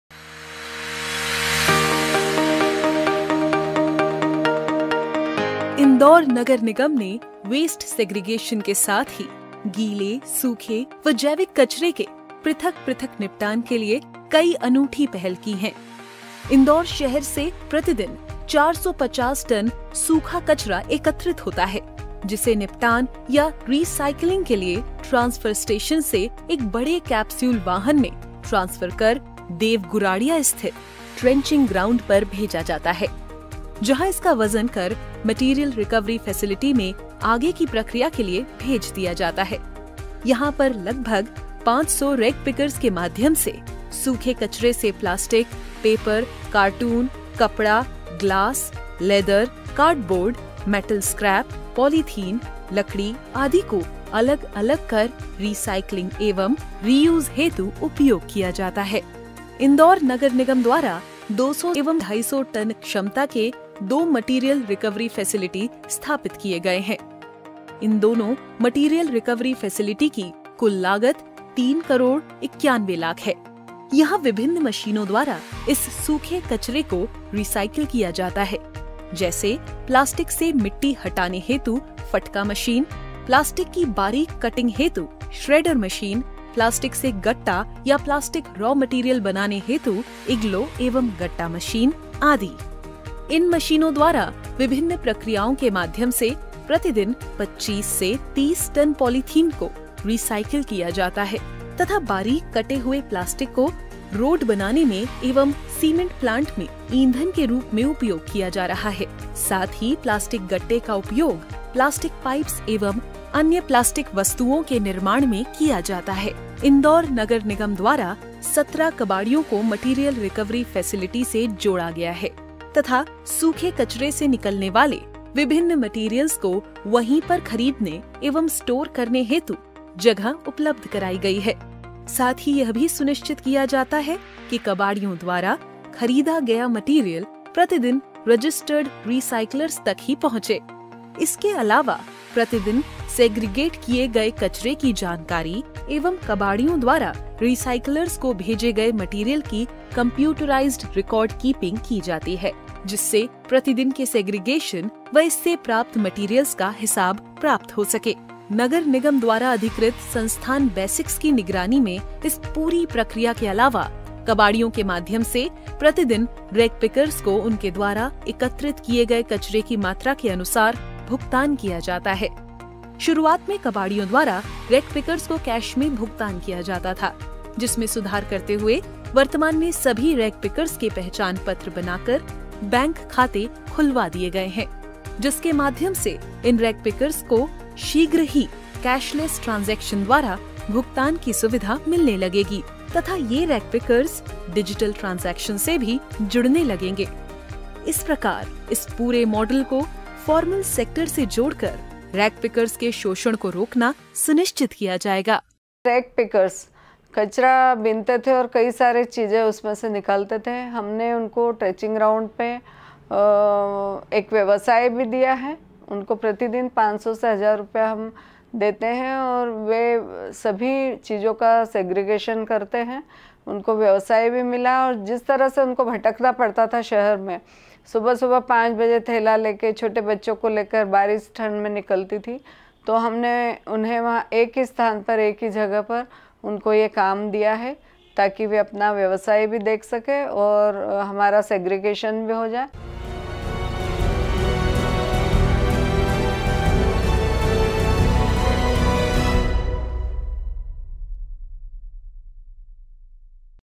Material Recovery Facility at Cleanest City - Indore
Disposal of dry waste by Material Recovery facility at Indore